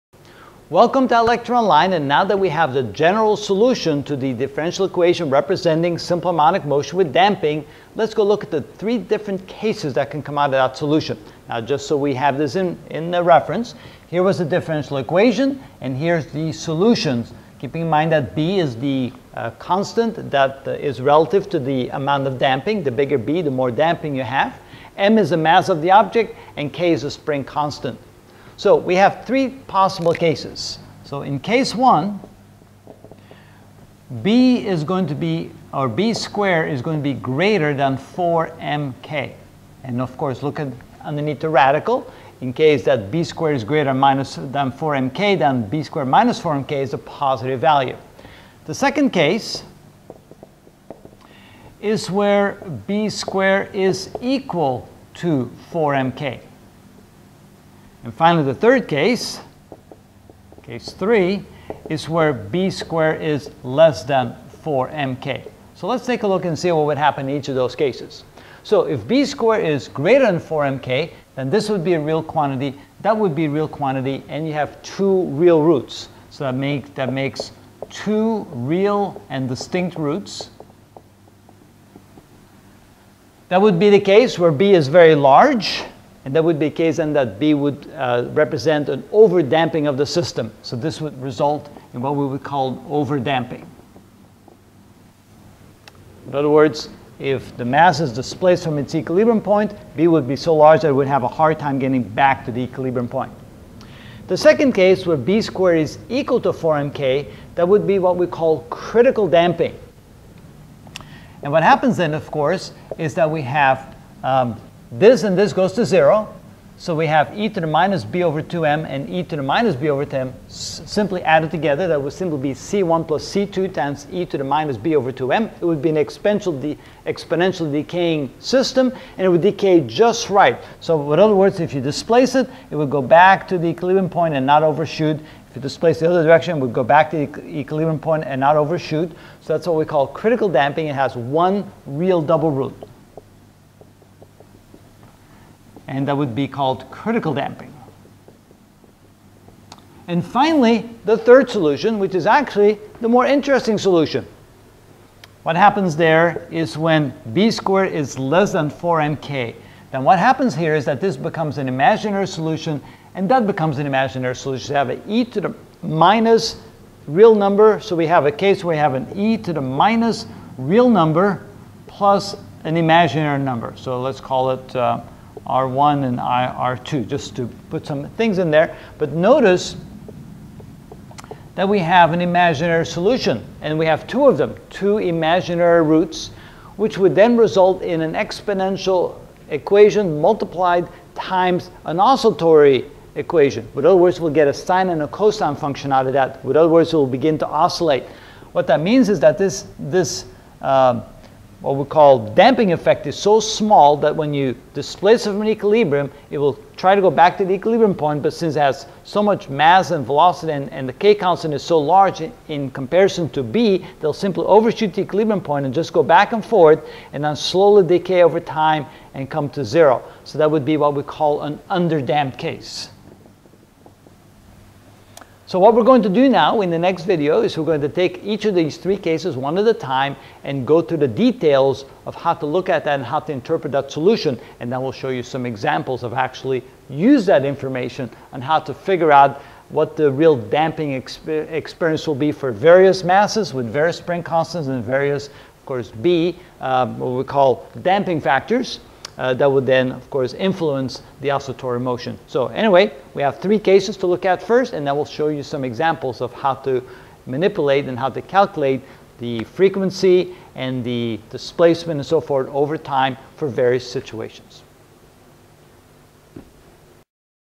Physics CH 16.1 Simple Harmonic Motion with Damping (13 of 20) The General Solution: The 3 Cases
In this video I will discuss the 3 cases of the general solution.